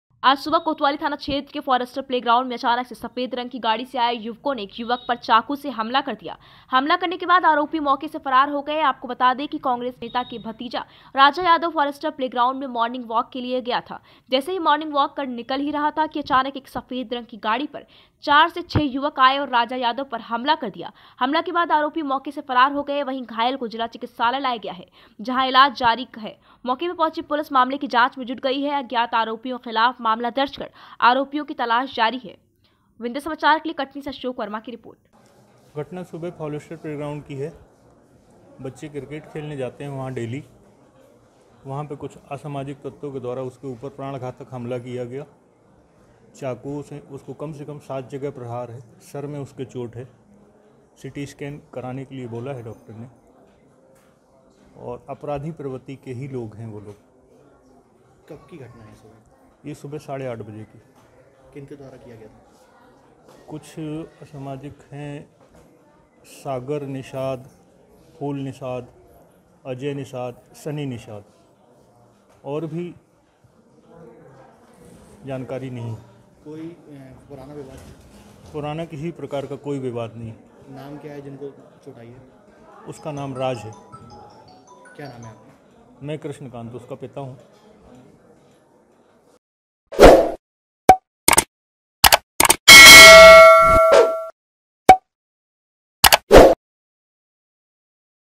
कांग्रेस नेता के भतीजे पर चाकू से हमलाvindhyasamachar.live
आज सुबह कोतवाली थाना क्षेत्र के फॉरेस्टर प्ले ग्राउंड में अचानक से सफेद रंग की गाड़ी से आए युवकों ने एक युवक पर चाकू से हमला कर दिया हमला करने के बाद आरोपी मौके से फरार हो गए आपको बता दें कि कांग्रेस नेता  के भतीजा राजा यादव फॉरेस्टर प्ले ग्राउंड में मॉर्निंग वॉक के लिए गया था जैसे ही मॉर्निंग वॉक  कर निकला ही रहा था कि अचानक एक सफेद रंग की गाड़ी पर चार से छह युवक आए और राजा यादव पर हमला कर दिया हमला के बाद आरोपी मौके से फरार हो गए वहीं घायल को जिला चिकित्सालय लाया गया जहां इलाज जारी मौके में पहुंची पुलिस मामले की जांच में जुटी अज्ञात आरोपियों के खिलाफ मामला दर्ज कर आरोपियों की तलाश जारी है।